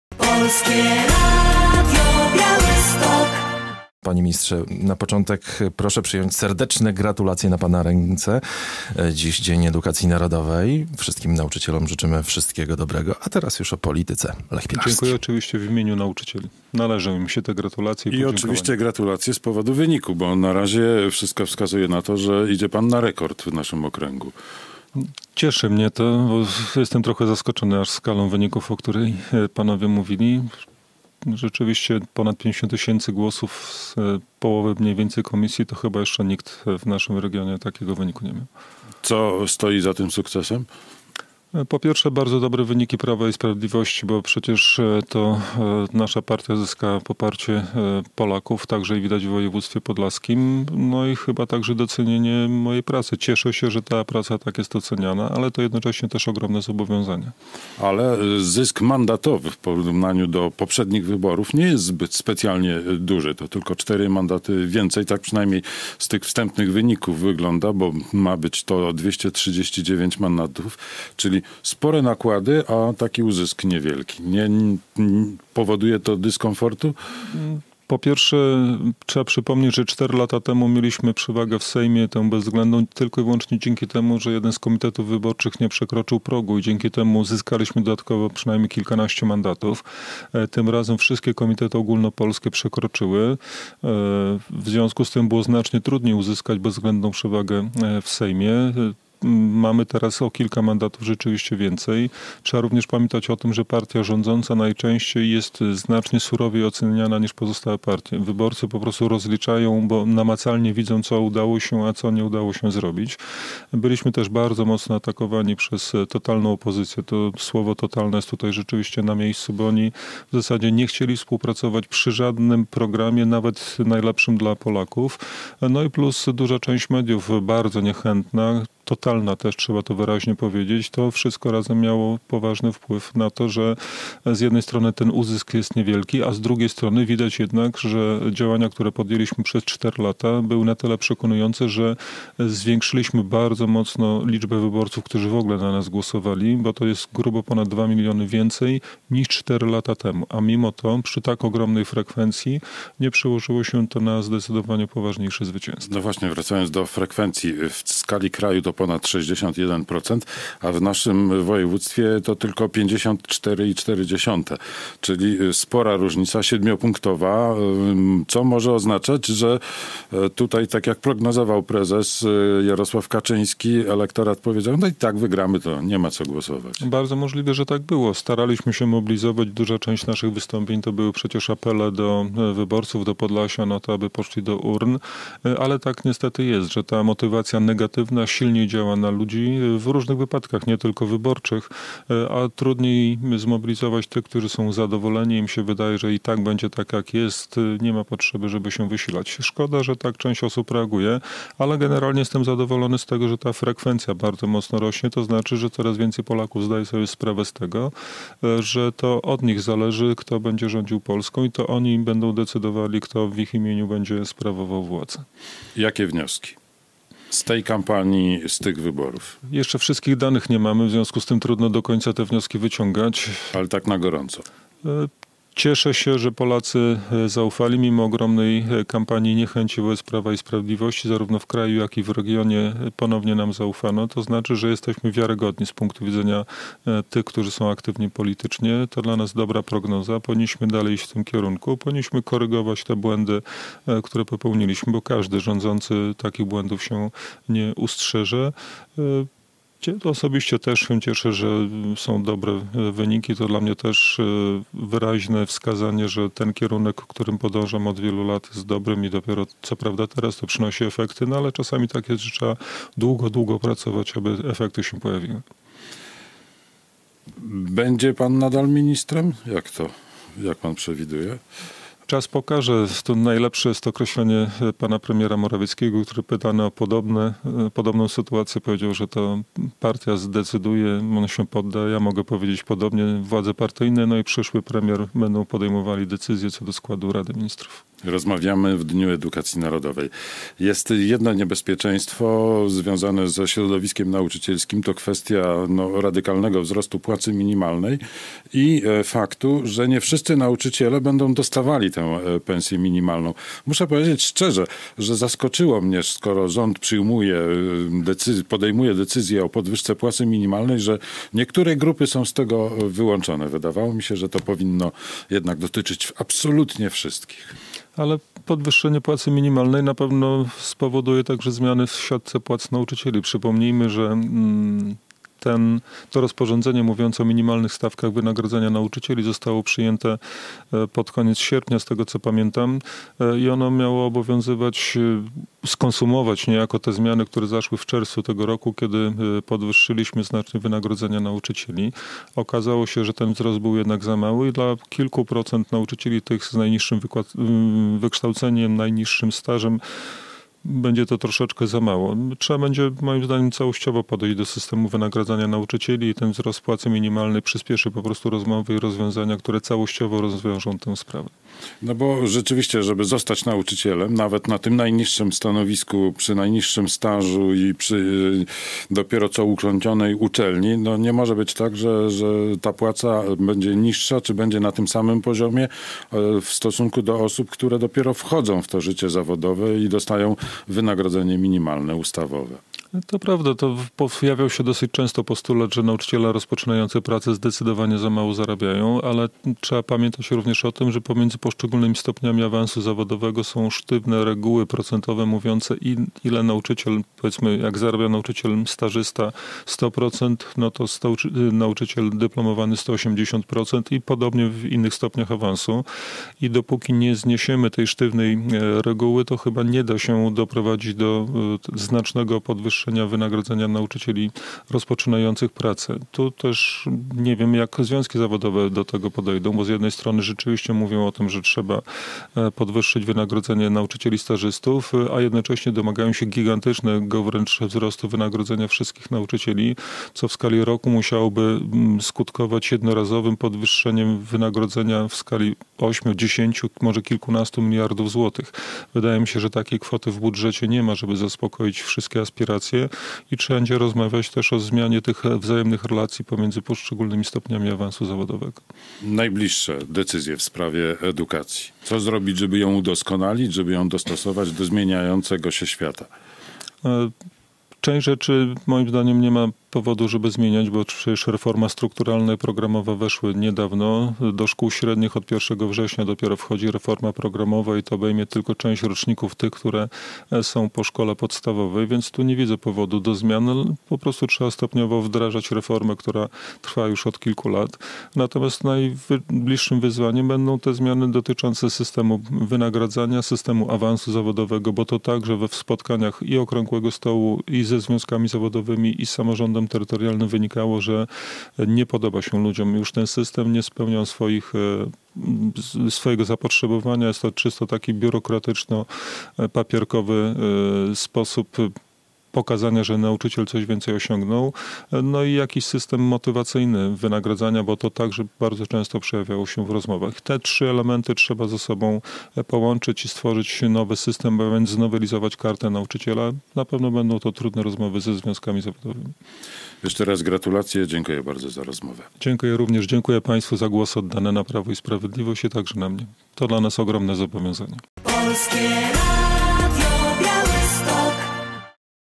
Poseł Dariusz Piontkowski o wynikach wyborów parlamentarnych 2019
Poseł PiS Dariusz Piontkowski o wynikach wyborów parlamentarnych 2019. Prawo i Sprawiedliwość zdeklasowało inne komitety wyborcze w województwie podlaskim - według sondażu late poll przygotowanego przez Ipsos zdobyło w regionie 53 proc. głosów.

Najwięcej osób zagłosowało na lidera PiS w Podlaskiem Dariusza Piontkowskiego - blisko 50 tys. (dane z 50.5 proc. obwodowych komisji wyborczych).

Z Dariuszem Piontkowskim rozmawia Lech Pilarski.

- Jestem zadowolony z tego, że frekwencja bardzo mocno rośnie. To znaczy, że coraz więcej Polaków zdaje sobie sprawę z tego, że to od nich zależy, kto będzie rządził Polską, i to oni będą decydowali, kto w ich imieniu będzie sprawował władzę - mówi Dariusz Piontkowski.